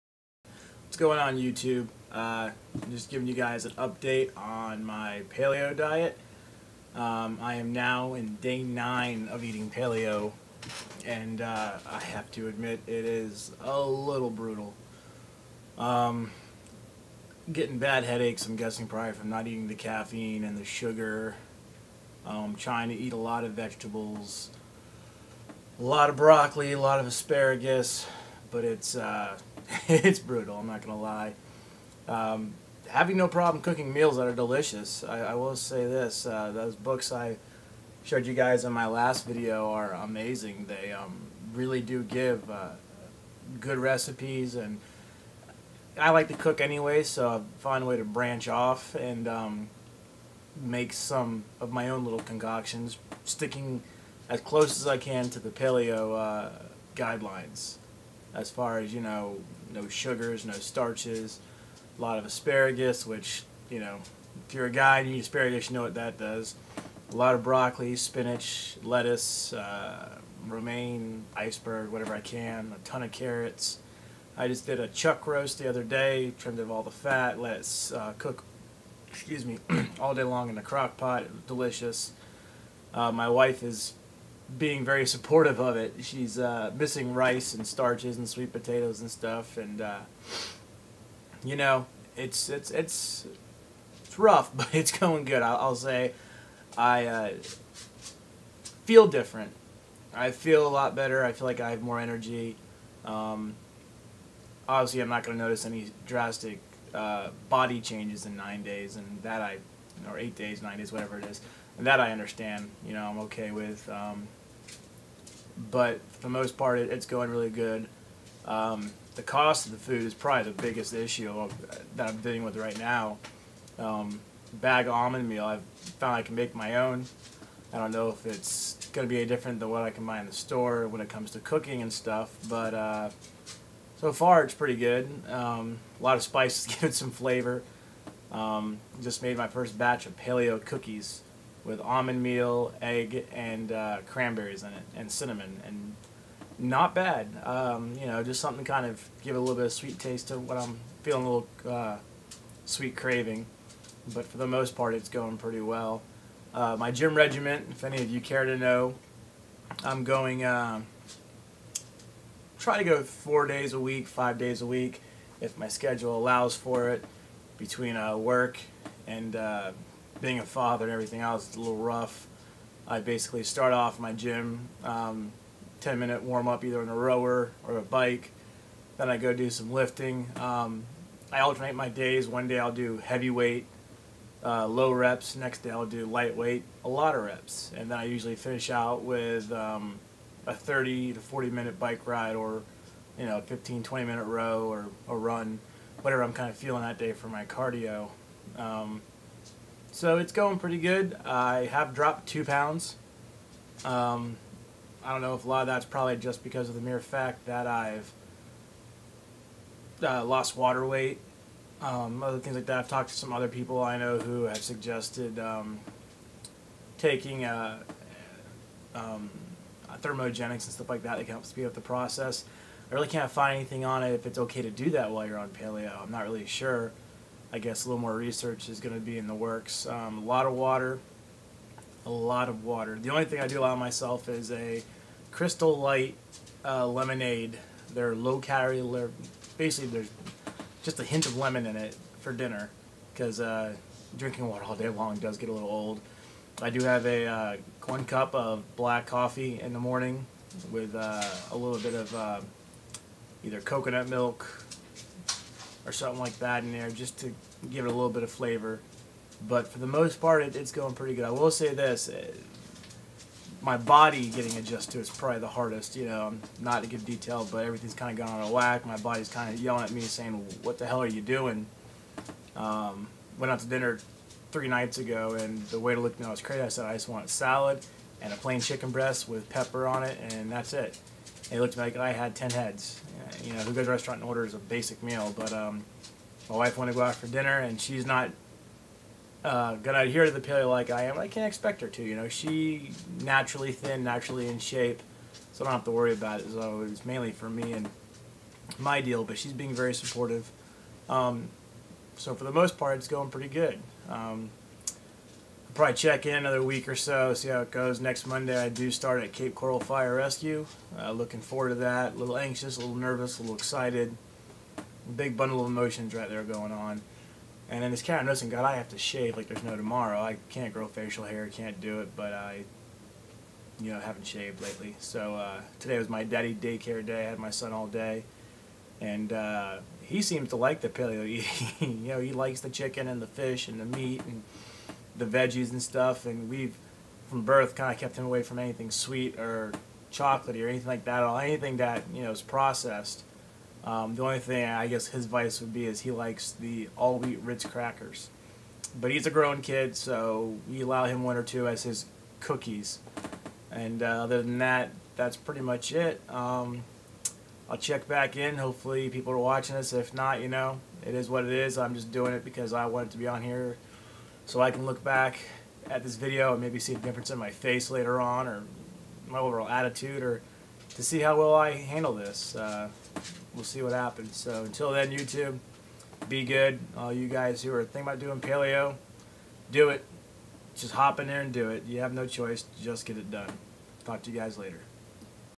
paleo3.mov
just a update on day 9 of eating paleo, will admit it is a lil tough going through sugar craving but all in all I can say I feel I am doing well